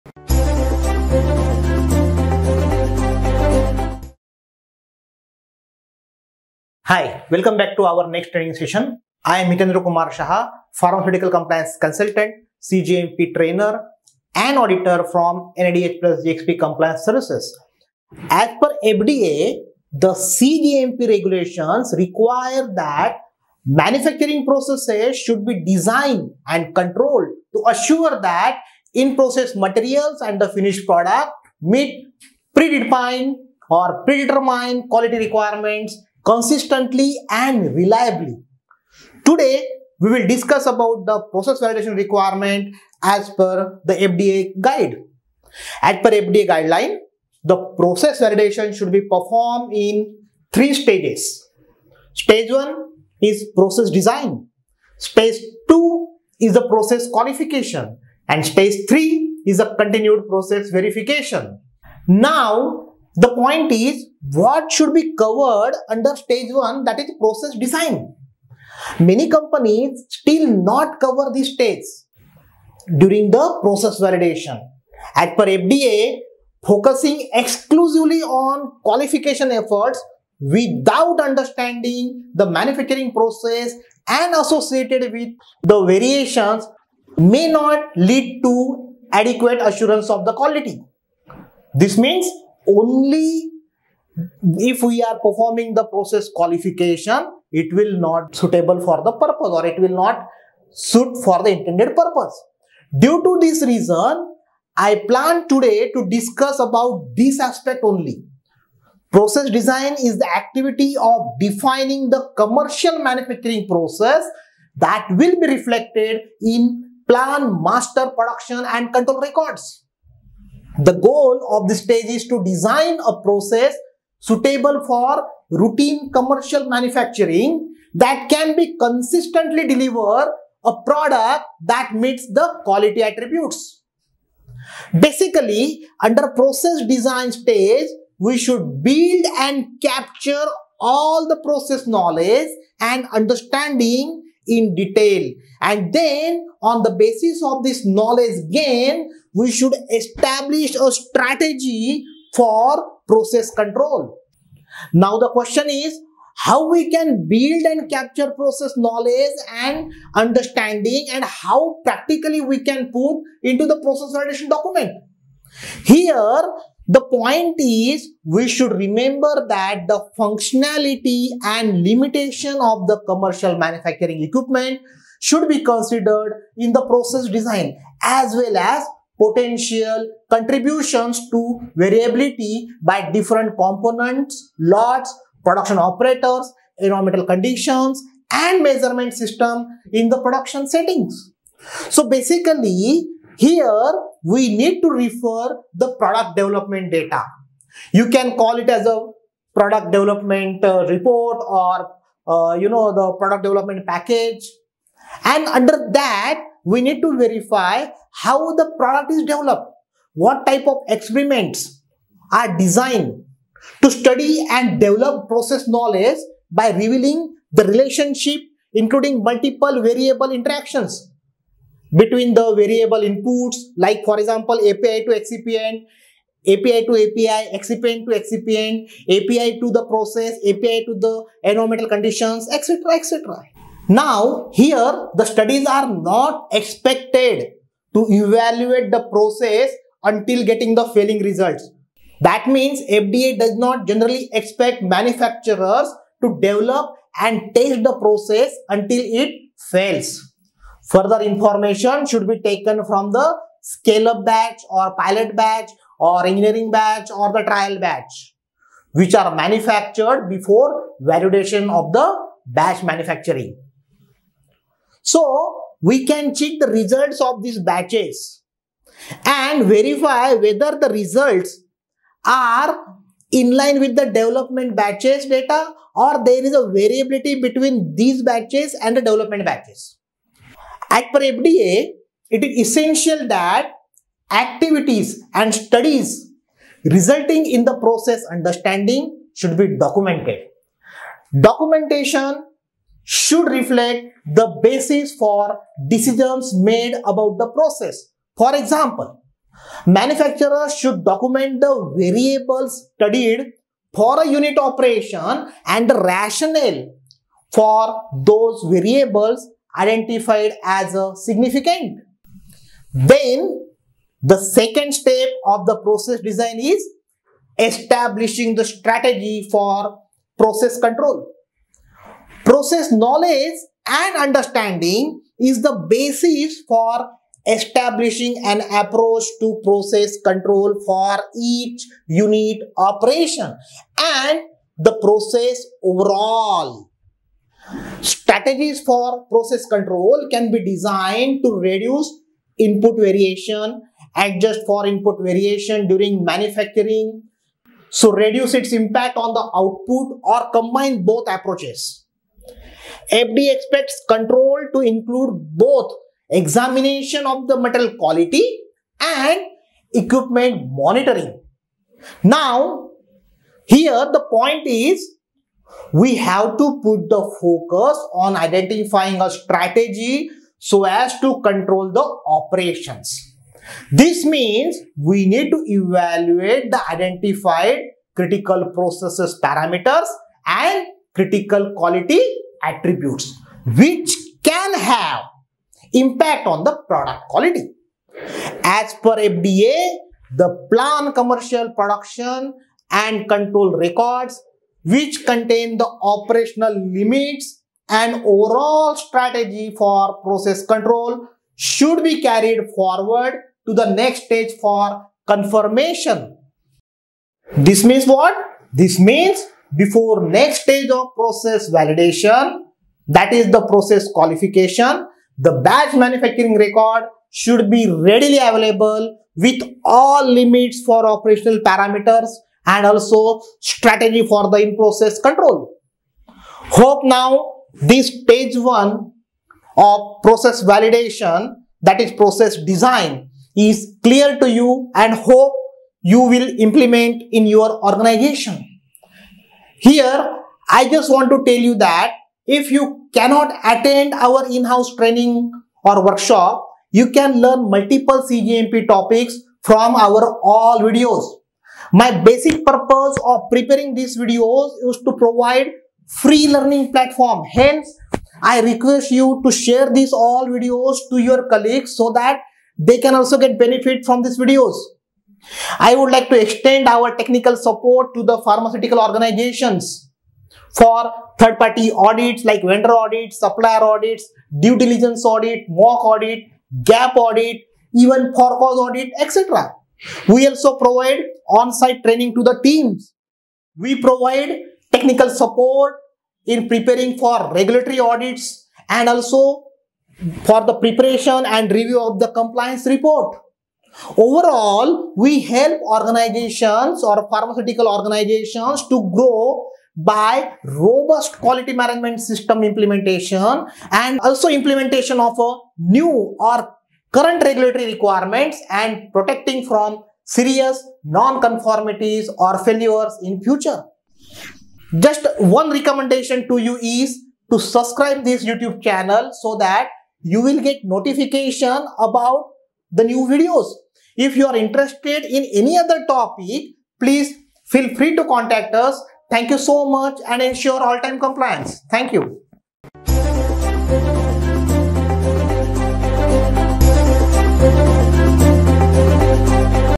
PROCESS VALIDATION STAGE-1 "PROCESS DESIGN"
This video helps viewers to understand and practically implement stage-1 of process validation. Many companies not implementing this stage during process validation. So, this video is created to make all understand how to practically implement the Process design during process validation.